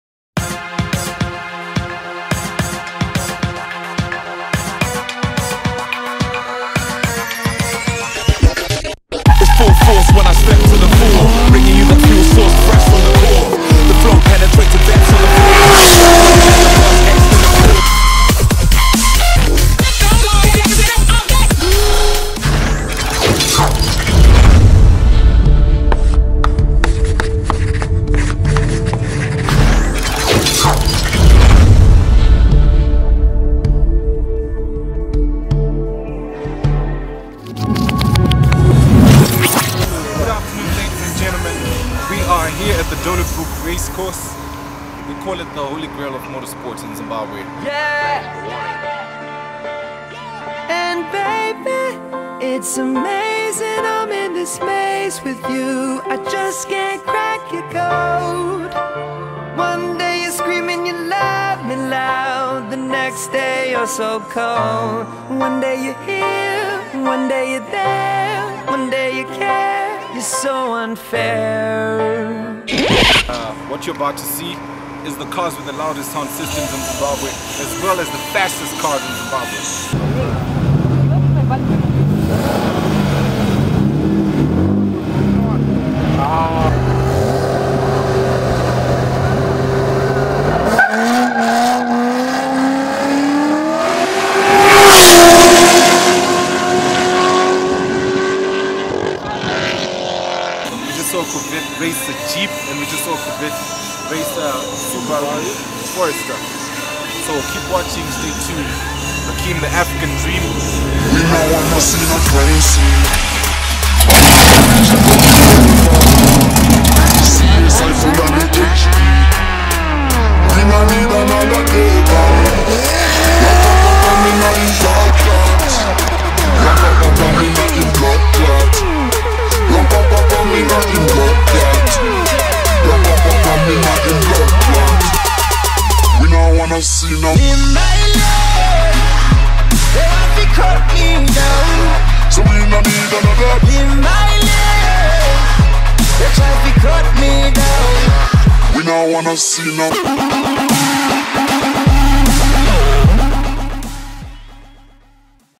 ZimtorqueTv Official Trailer and Launch!!!
2014 will see the online launch of the most unbelievable zimbabwean cars and driving experience you can never imagine